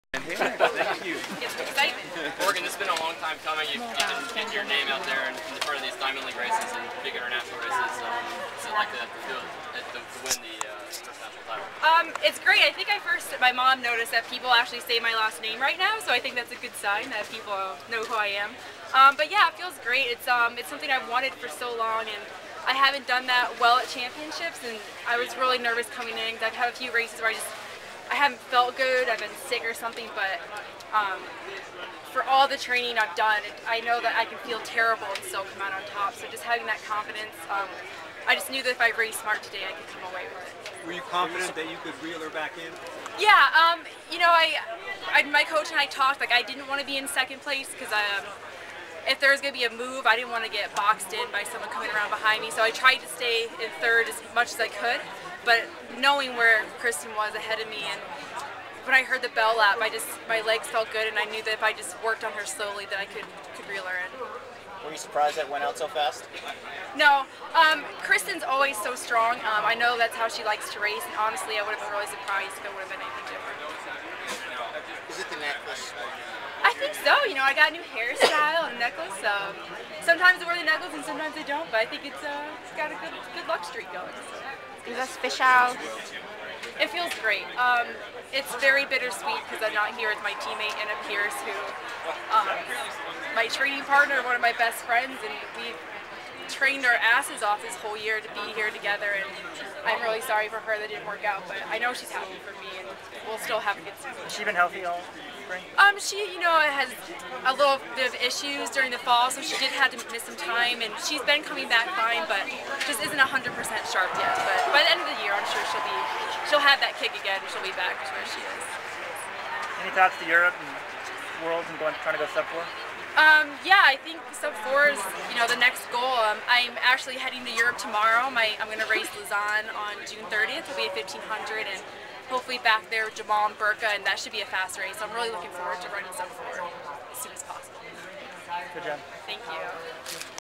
Morgan Uceny After Winning Her First USATF 1500m Title
for more coverage from the 2011 USATF Nationals in Eugene, Oregon. Uceny caught Christin Wurth-Thomas to convincingly to win the title as Jenny Simpson was second and Shannon Rowbury third.